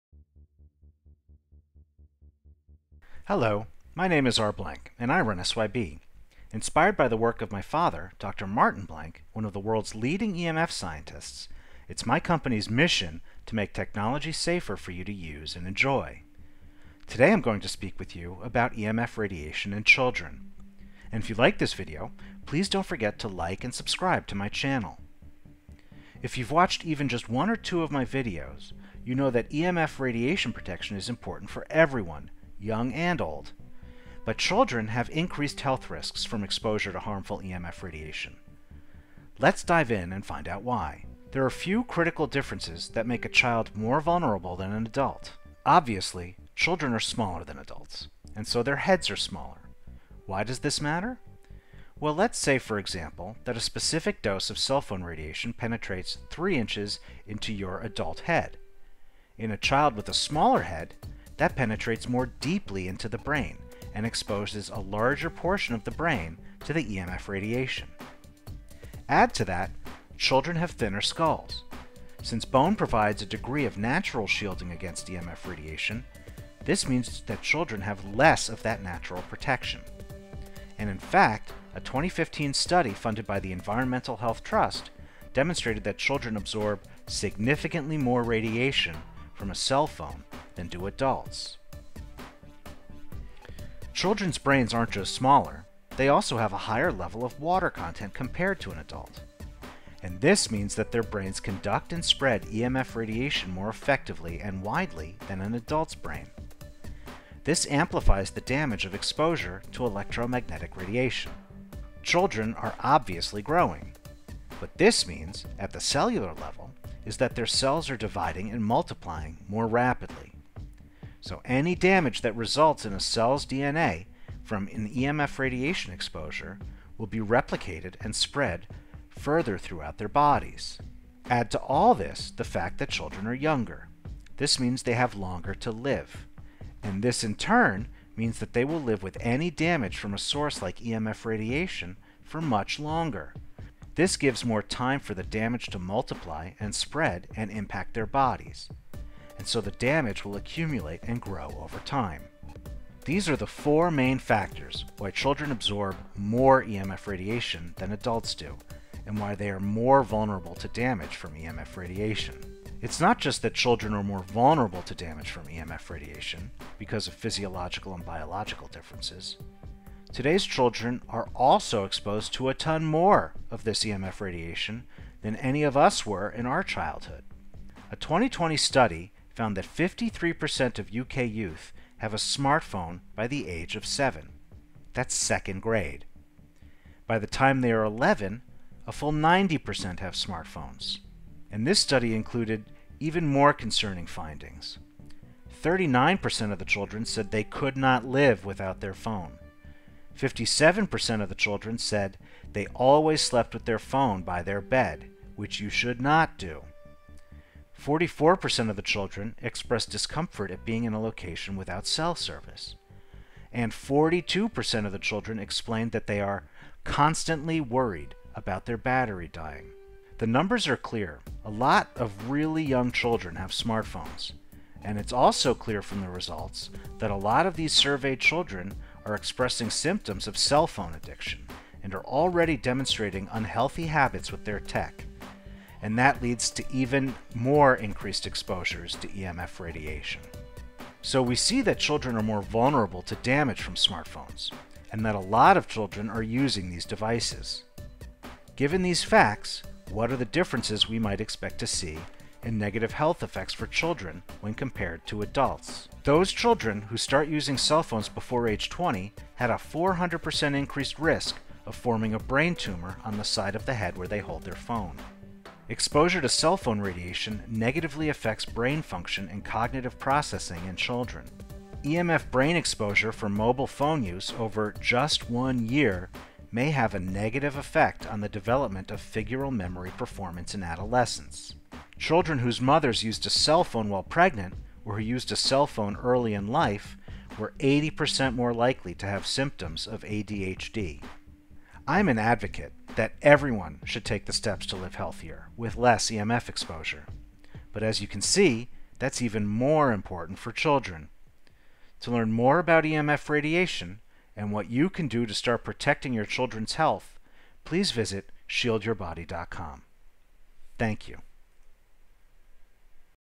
The Health Effects of EMF Radiation on Children
EMF radiation protection is important for everyone, young and old. But children have increased health risks from exposure to harmful EMF radiation. In this video, I explain why children are more vulnerable, and what some of the known health effects are.